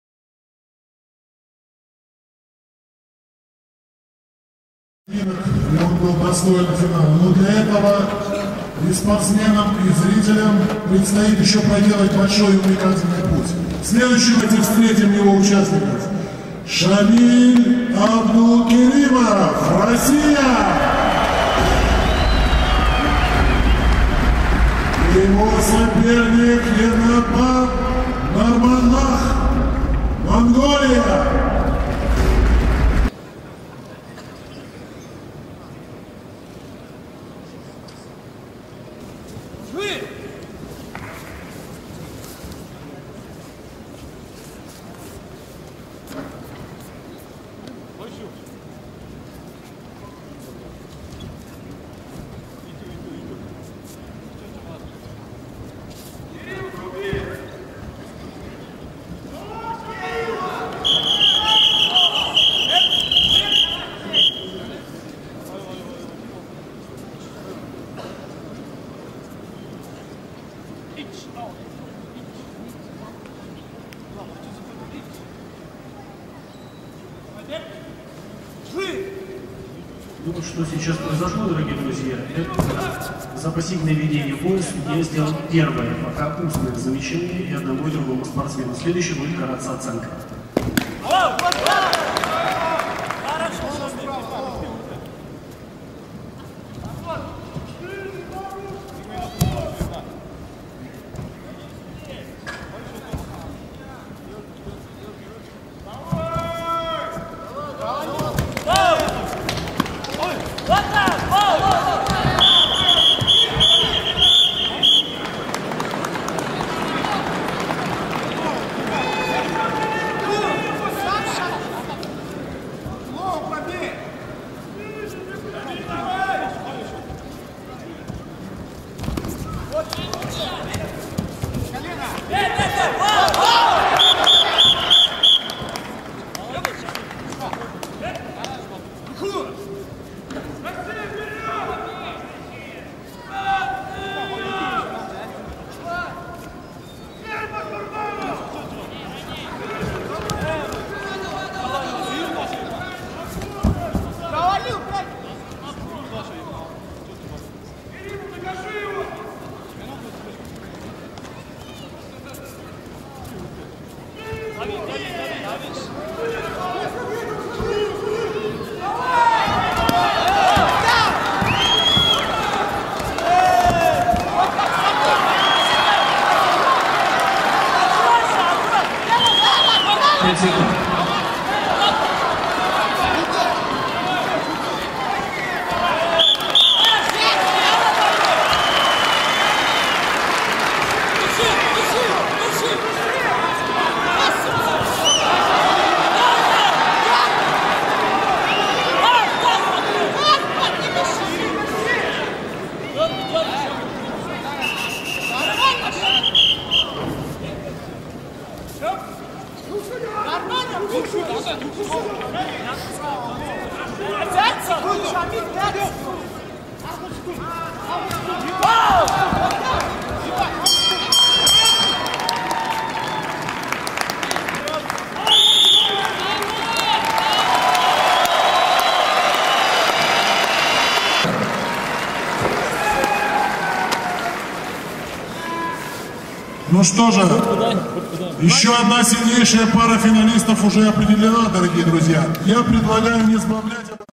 KUDO World Cup 2011. Abdulkerimov (Russia) vs Narmandakh (Mongolia), Division
KUDO World Cup 2011. Abdulkerimov (Russia) vs Narmandakh (Mongolia), Division 240, SemiFINAL.
КУДО Кубок Мира 2011.  Abdulkerimov (Russia) vs Narmandakh (Mongolia), Категория 240, Полуфинал.